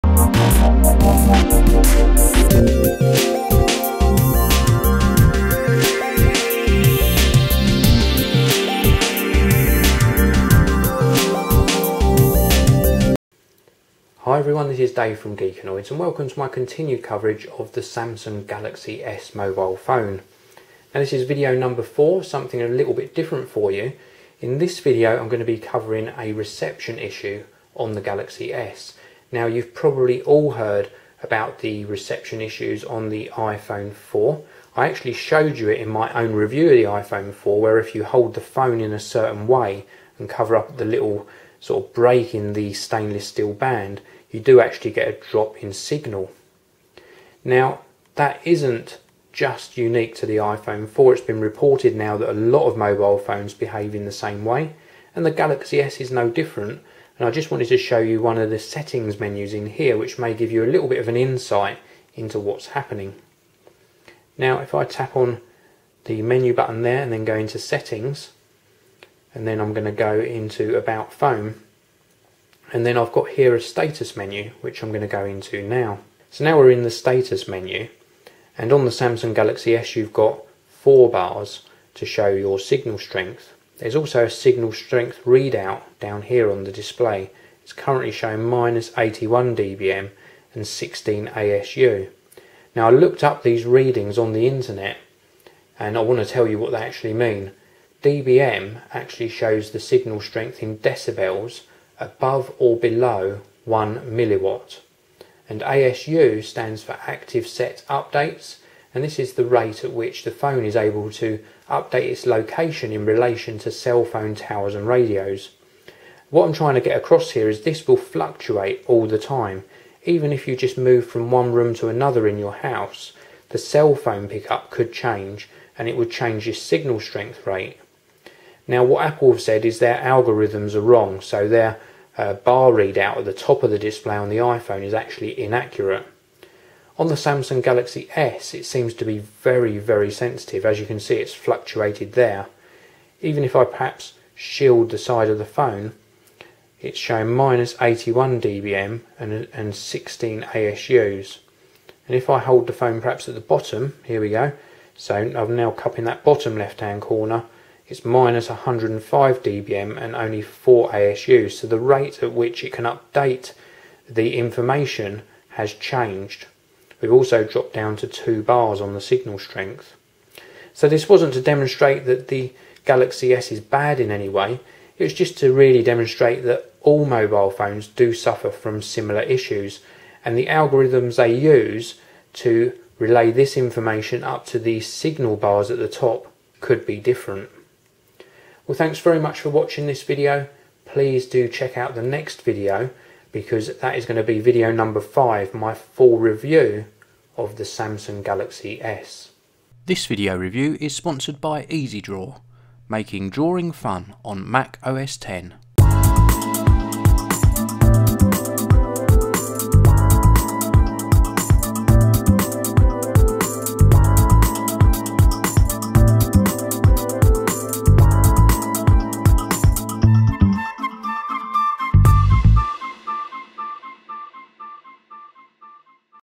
Samsung Galaxy S (Vibrant) - Video 4 - Signal Reception
Samsung Galaxy S - Video 4. I have published this video, not to show you that the Samsung Galaxy S has any signal problem (like the iPhone 4), but to show you that all mobile phones are susceptible to fluctuations in signal strength. I also take a look at the menu on the Galaxy S that show the dBm and asu readings.